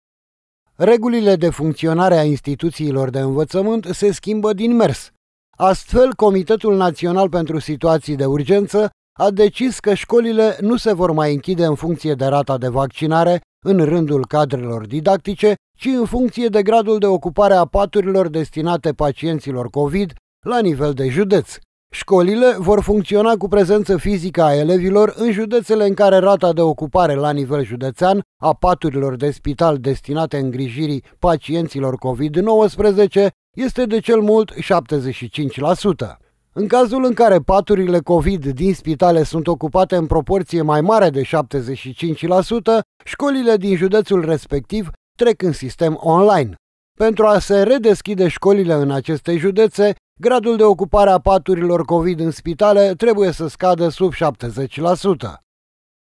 Brambureala din sistemul de învăţământ continuă ( 14 01 2022 )
Scurtă descriere:

Măsurile Comitetului pentru Situaţii de Urgenţă, în privinţa desfăşurării activităţii din instituţiile de învăţământ, se schimbă de la o zi la alta. Conform Comitetului, de data aceasta, şcolile se vor închide în funcţie de gradul de ocupare a paturilor în spitalele COVID, la nivel de judeţ. Dacă acestea sunt ocupate în proporţie de 75%, se va trece la predarea online.